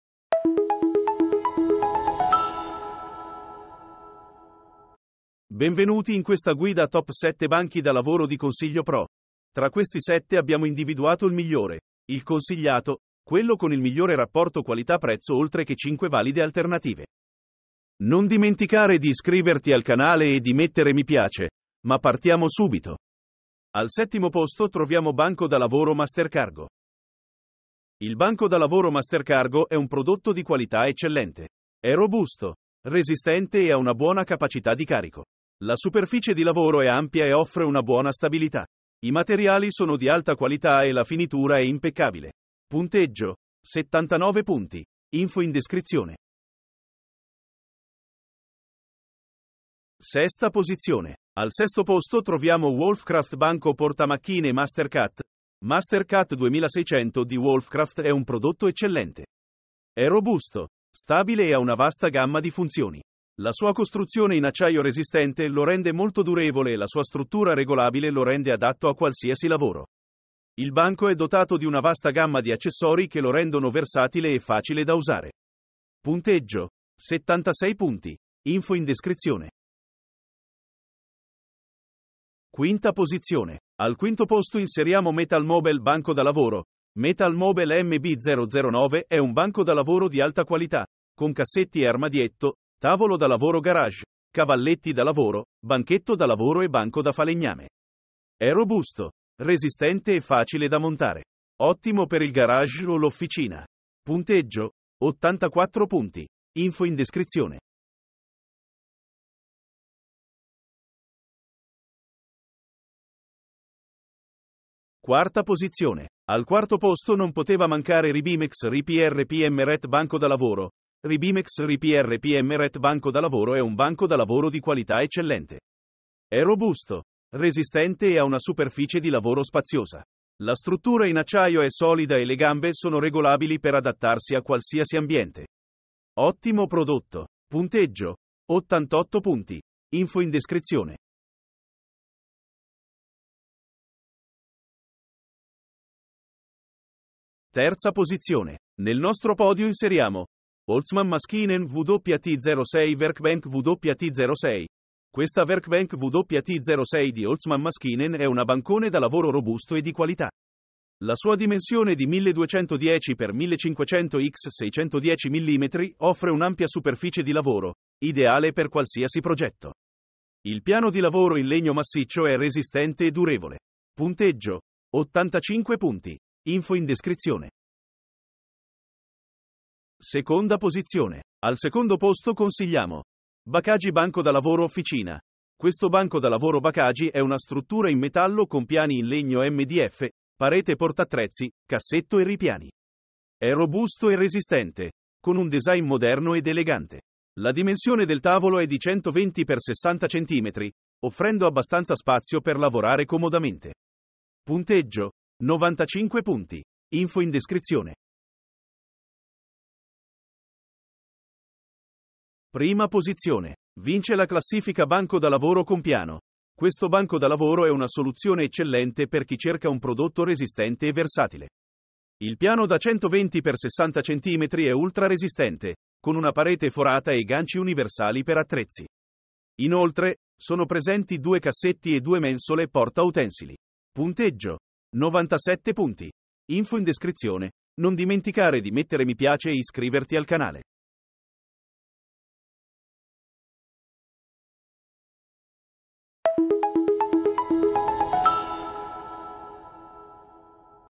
Migliori banchi da lavoro Qualità Prezzo 2023
🔥 Nome Articolo: I Migliori Banchi da Lavoro - Guida Acquisto e Top 10 Perfetti!
Non sai quale banco da lavoro acquistare? Segui i nostri consigli sui migliori banchi da lavoro 2023 con la guida acquisto e top 10 perfetti
👉 Per vedere i prezzi e altre foto, ecco i link qui sotto
Top sette migliori banchi da lavoro del 2023 di ConsiglioPro.it
► 1° in classifica Banco da Lavoro con piano
► 2° in classifica Bakaji Banco da Lavoro Officina
► 3° in classifica Holzmann Maschinen WT06 Werkbank WT06
► 4° in classifica Ribimex RI-PRPMRET Banco da Lavoro
► 5° in classifica: METALLMOBELL - Banco da Lavoro
► 6° in classifica wolfcraft Banco portamacchine MASTER cut
► 7° in classifica: Banco da lavoro MasterCargo

Hai bisogno di un consiglio personale? Commenta o visita il nostro sito

⏱🚨🚛 ⎮Prova Prime Gratis 30 gg (Spedizione gratuita e CONSEGNA IN 24 ORE!
Come? Qui